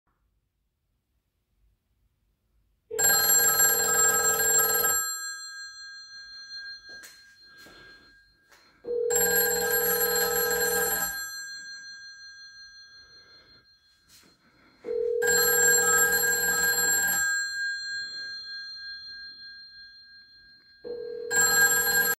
Rotary Wall Phone Ringing
This is my 1970's rotary wall phone.  It is fully operational using a VOIP converter.